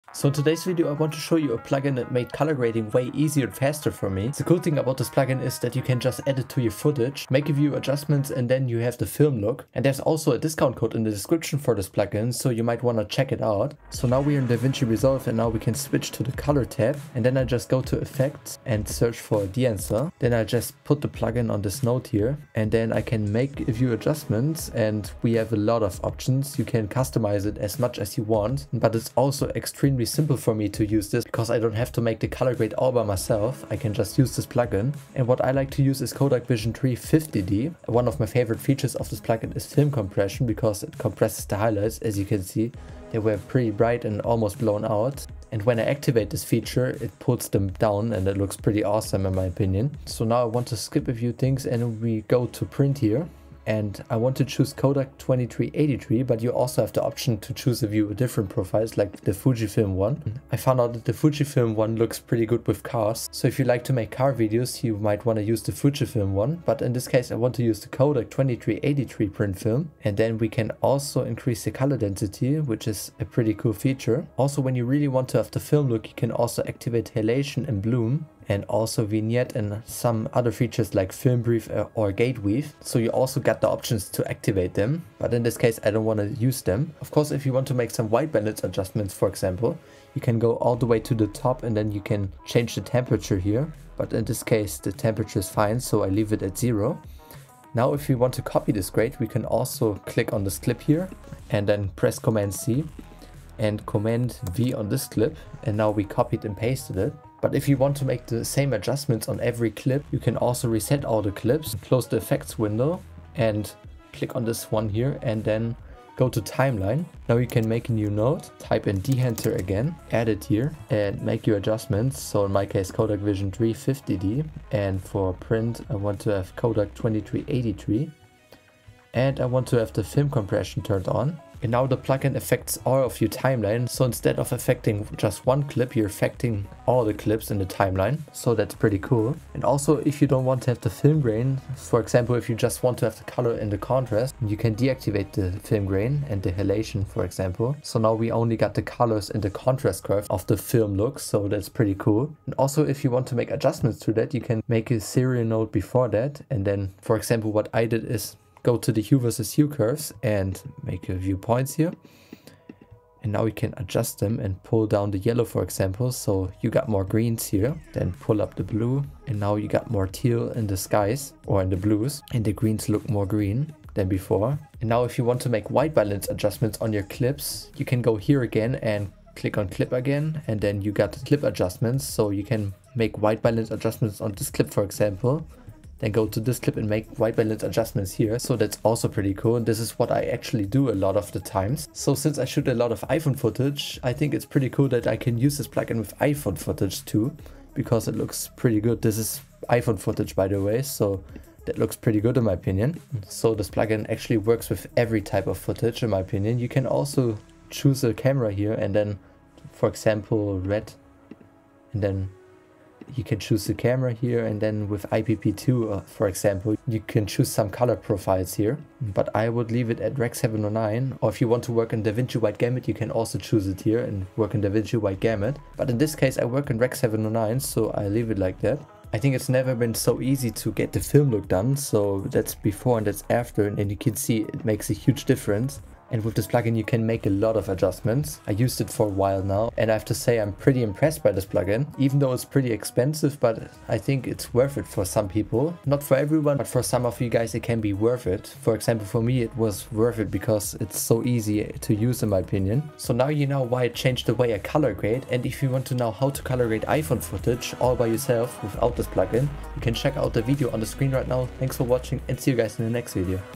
This Plugin Will Make Your Color Grading Stand Out!
In this Video I show you my Workflow with a Plugin called Dehancer, that saves me a lot of time with Color Grading in Davinci Resolve!

My Equipment:
- iPhone 12*
- Rode VideoMicro*
- MacBook Pro M1 (2020)*
- Sony ZV-E10*

2023 MBJ Media